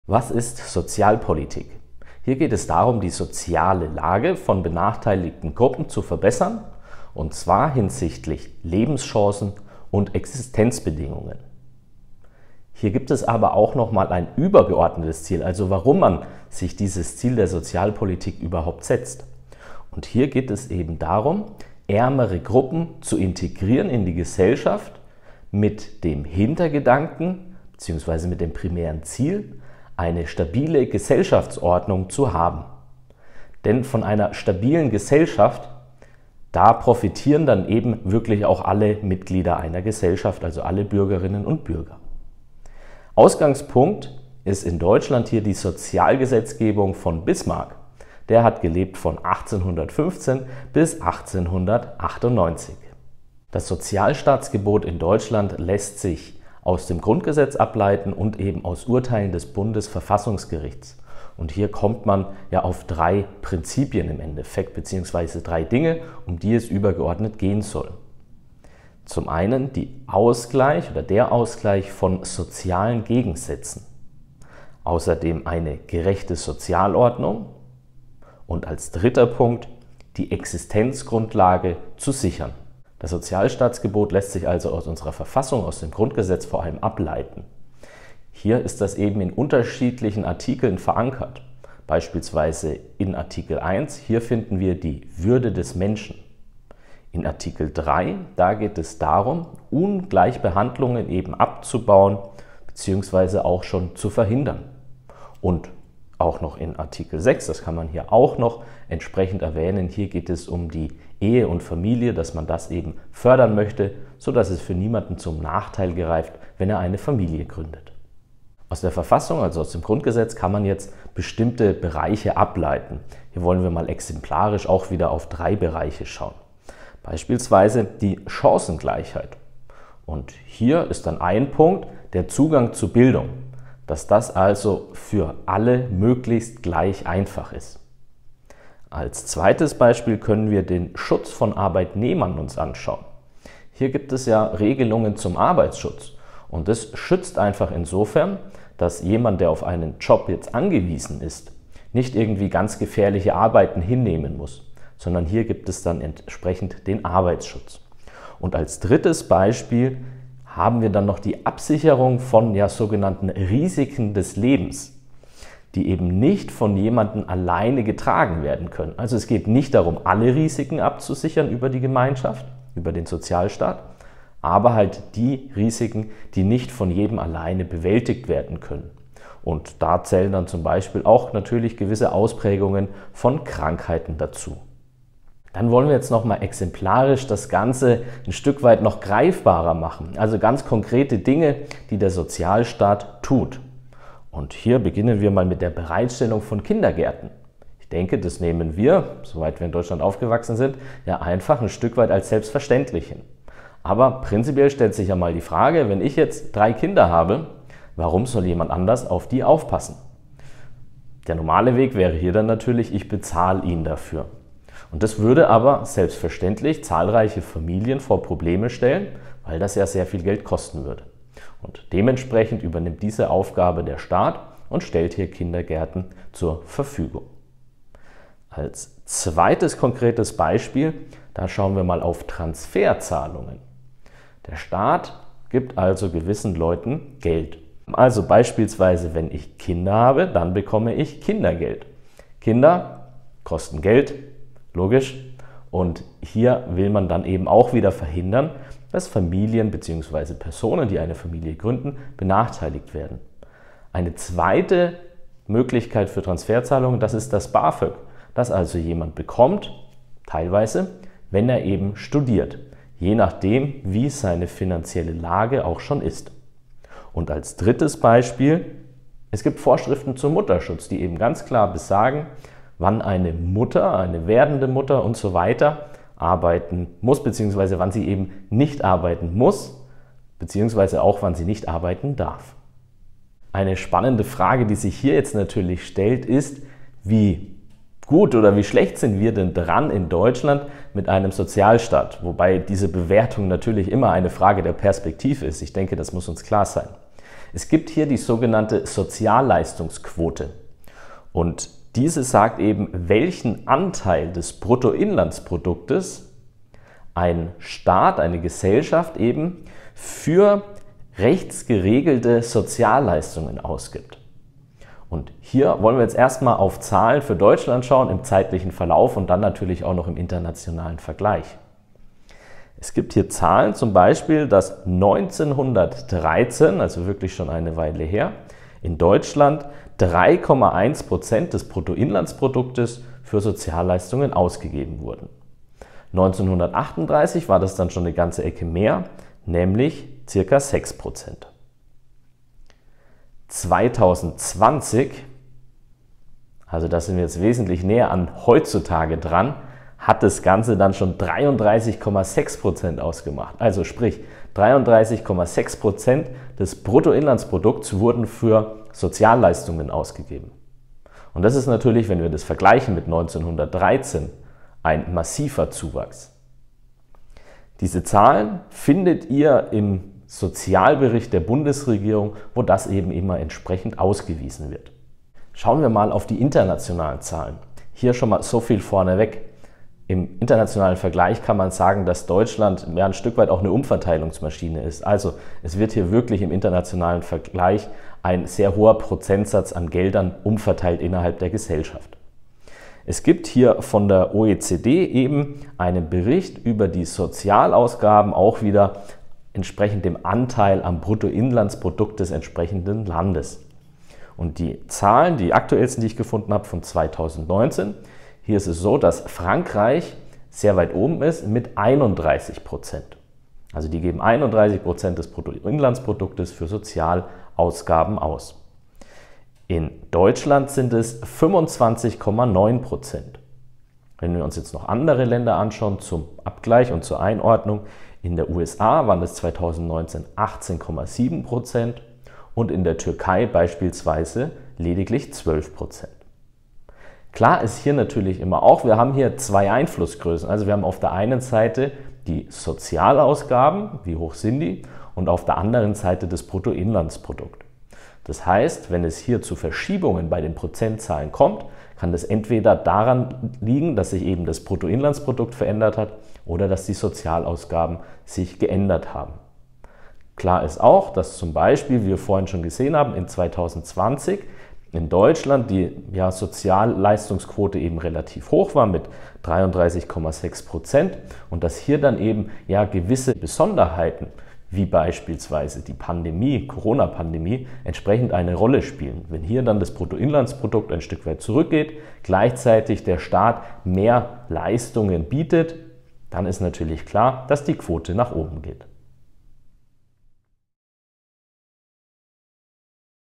Was ist Sozialpolitik?
Sozialpolitik einfach erklärt, inklusive Sozialleistungsquote. Die soziale Lage benachteiligter Gruppen soll durch Angleichung von Lebenschancen und Existenzbedingungen verbessert werden. Übergeordnetes Ziel ist eine stabile Gesellschaftsordnung. Ausgangspunkt ist die Sozialgesetzgebung Bismarcks. Das Sozialstaatsgebot hat nach Grundgesetz und Urteilen des Bundesverfassungsgerichts mehrere Ziele. Der Ausgleich sozialer Gegensätze, eine gerechte Sozialordnung und die Sicherung der Existenzgrundlage. Die Sozialleitungsquote findet man im Sozialbericht der Bundesregierung. Der internationale Vergleich ist mit Daten der OECD möglich. Diese vergleicht den Anteil der Sozialausgaben am Bruttoinlandsprodukt (BIP) für unterschiedliche Staaten.